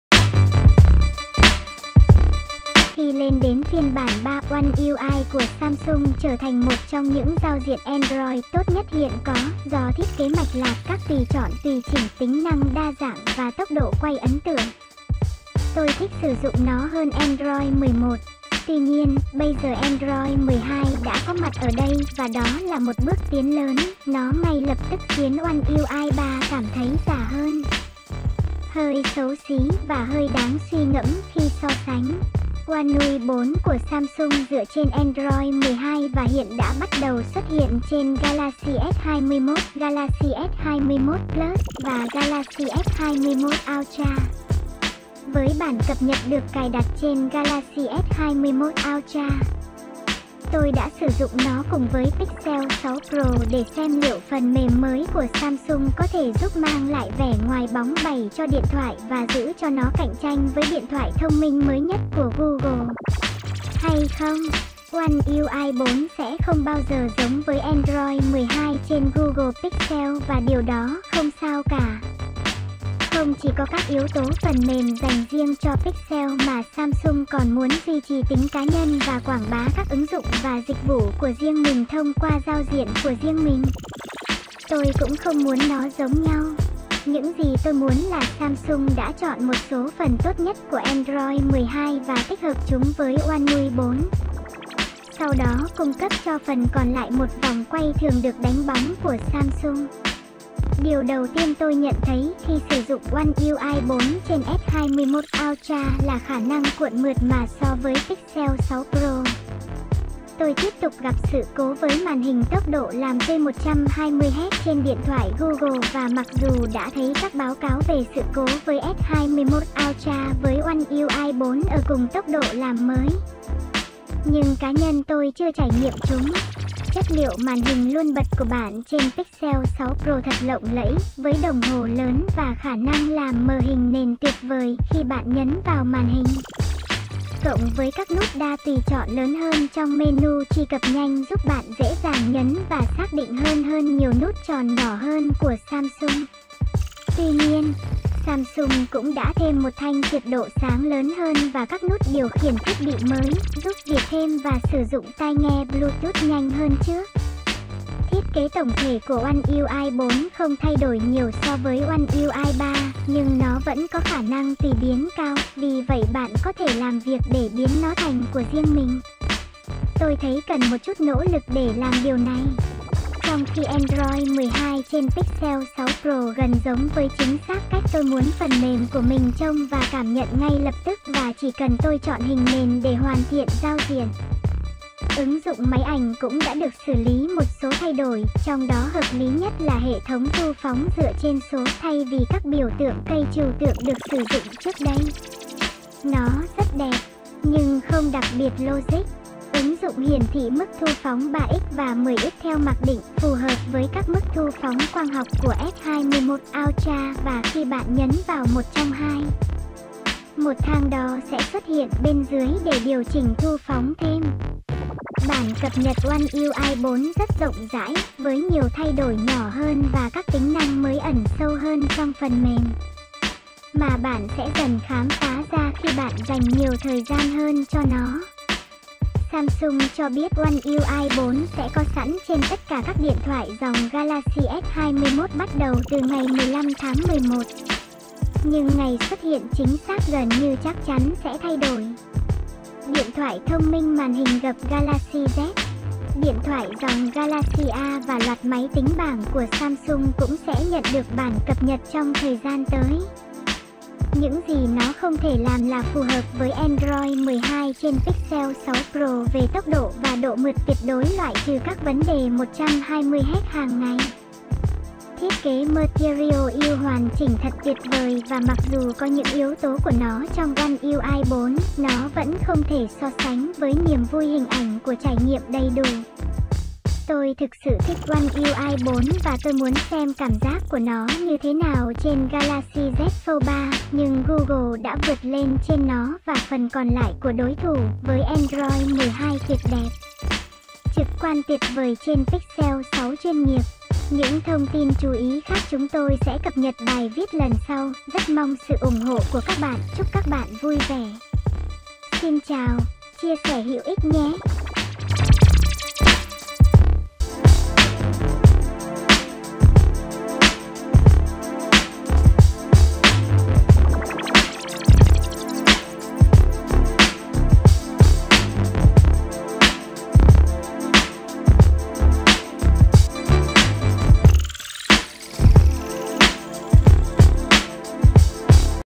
So sánh One UI 4 của Samsung với Android 12 trên Google Pixel 6 Pro
Khi lên đến phiên bản 3, One UI của Samsung trở thành một trong những giao diện Android tốt nhất hiện có, do thiết kế mạch lạc, các tùy chọn tùy chỉnh, tính năng đa dạng và tốc độ quay ấn tượng. Tôi thích sử dụng nó hơn Android 11. Tuy nhiên, bây giờ Android 12 đã có mặt ở đây và đó là một bước tiến lớn, nó ngay lập tức khiến One UI 3 cảm thấy già hơn, hơi xấu xí và hơi đáng suy ngẫm khi so sánh.

OneUI 4 của Samsung dựa trên Android 12 và hiện đã bắt đầu xuất hiện trên Galaxy S21, Galaxy S21 Plus và Galaxy S21 Ultra. Với bản cập nhật được cài đặt trên Galaxy S21 Ultra , tôi đã sử dụng nó cùng với Pixel 6 Pro để xem liệu phần mềm mới của Samsung có thể giúp mang lại vẻ ngoài bóng bẩy cho điện thoại và giữ cho nó cạnh tranh với điện thoại thông minh mới nhất của Google hay không.

One UI 4 sẽ không bao giờ giống với Android 12 trên Google Pixel và điều đó không sao cả. Không chỉ có các yếu tố phần mềm dành riêng cho Pixel mà Samsung còn muốn duy trì tính cá nhân và quảng bá các ứng dụng và dịch vụ của riêng mình thông qua giao diện của riêng mình. Tôi cũng không muốn nó giống nhau. Những gì tôi muốn là Samsung đã chọn một số phần tốt nhất của Android 12 và tích hợp chúng với OneUI 4, sau đó cung cấp cho phần còn lại một vòng quay thường được đánh bóng của Samsung.

Điều đầu tiên tôi nhận thấy khi sử dụng One UI 4 trên S21 Ultra là khả năng cuộn mượt mà so với Pixel 6 Pro. Tôi tiếp tục gặp sự cố với màn hình tốc độ làm tươi 120Hz trên điện thoại Google và mặc dù đã thấy các báo cáo về sự cố với S21 Ultra với One UI 4 ở cùng tốc độ làm mới, nhưng cá nhân tôi chưa trải nghiệm chúng.

Chất liệu Màn hình luôn bật của bạn trên Pixel 6 Pro thật lộng lẫy, với đồng hồ lớn và khả năng làm mờ hình nền tuyệt vời khi bạn nhấn vào màn hình, cộng với các nút đa tùy chọn lớn hơn trong menu Truy cập nhanh giúp bạn dễ dàng nhấn và xác định hơn hơn nhiều nút tròn nhỏ hơn của Samsung. Tuy nhiên, Samsung cũng đã thêm một thanh trượt độ sáng lớn hơn và các nút điều khiển thiết bị mới, giúp việc thêm và sử dụng tai nghe Bluetooth nhanh hơn trước.

Thiết kế tổng thể của One UI 4 không thay đổi nhiều so với One UI 3, nhưng nó vẫn có khả năng tùy biến cao, vì vậy bạn có thể làm việc để biến nó thành của riêng mình. Tôi thấy cần một chút nỗ lực để làm điều này, trong khi Android 12 trên Pixel 6 Pro gần giống với chính xác cách tôi muốn phần mềm của mình trông và cảm nhận ngay lập tức và chỉ cần tôi chọn hình nền để hoàn thiện giao diện.

Ứng dụng máy ảnh cũng đã được xử lý một số thay đổi, trong đó hợp lý nhất là hệ thống thu phóng dựa trên số, thay vì các biểu tượng cây trừu tượng được sử dụng trước đây. Nó rất đẹp, nhưng không đặc biệt logic. Ứng dụng hiển thị mức thu phóng 3x và 10x theo mặc định, phù hợp với các mức thu phóng quang học của S21 Ultra và khi bạn nhấn vào một trong hai, một thang đo sẽ xuất hiện bên dưới để điều chỉnh thu phóng thêm.

Bản cập nhật One UI 4 rất rộng rãi, với nhiều thay đổi nhỏ hơn và các tính năng mới ẩn sâu hơn trong phần mềm, mà bạn sẽ dần khám phá ra khi bạn dành nhiều thời gian hơn cho nó. Samsung cho biết One UI 4 sẽ có sẵn trên tất cả các điện thoại dòng Galaxy S21 bắt đầu từ ngày 15 tháng 11, nhưng ngày xuất hiện chính xác gần như chắc chắn sẽ thay đổi. Điện thoại thông minh màn hình gập Galaxy Z, điện thoại dòng Galaxy A và loạt máy tính bảng của Samsung cũng sẽ nhận được bản cập nhật trong thời gian tới.

Những gì nó không thể làm là phù hợp với Android 12 trên Pixel 6 Pro về tốc độ và độ mượt tuyệt đối - loại trừ các vấn đề 120Hz - hàng ngày. Thiết kế Material You hoàn chỉnh thật tuyệt vời và mặc dù có những yếu tố của nó trong One UI 4, nó vẫn không thể so sánh với niềm vui hình ảnh của trải nghiệm đầy đủ. Tôi thực sự thích One UI 4 và tôi muốn xem cảm giác của nó như thế nào trên Galaxy Z Fold 3, nhưng Google đã vượt lên trên nó và phần còn lại của đối thủ, với Android 12 tuyệt đẹp, trực quan tuyệt vời trên Pixel 6 Chuyên nghiệp.